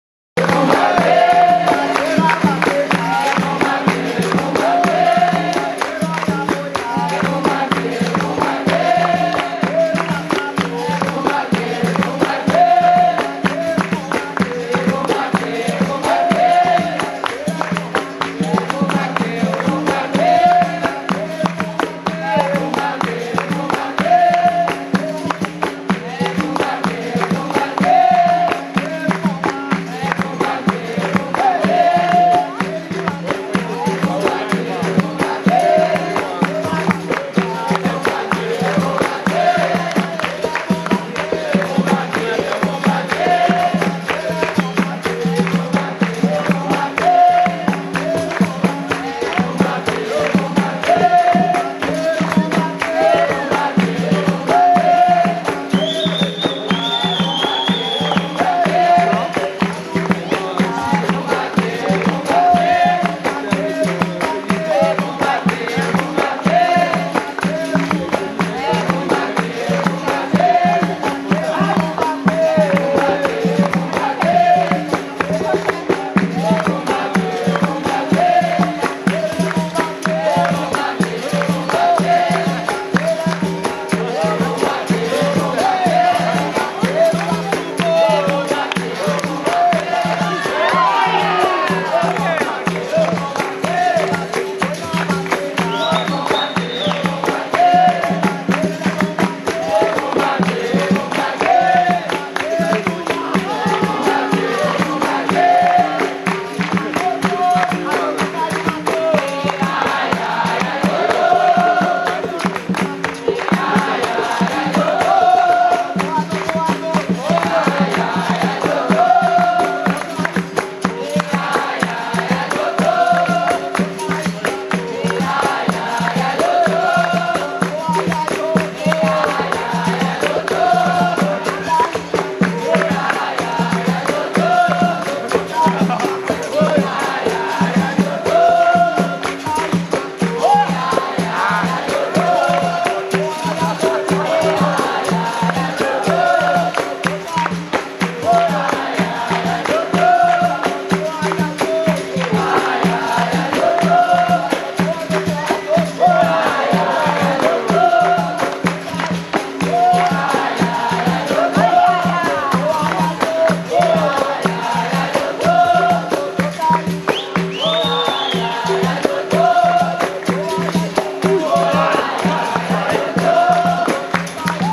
Inst Fofo - CM Ivan - CM Tico PAF 2015
Roda Passo a Frente 2015 jogam:
Inst Fofo - CM Ivan
CM Ivan - CM Tico
Mestre Calango puxando o canto